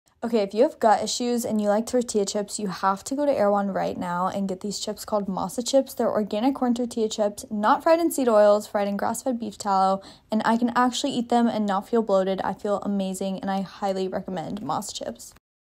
Seed oil free shopping at Erewhon
Finding snacks without seed oils can be tough, but not at Erewhon! Join us as we buy MASA chips and some other healthy snacks on our trip to LA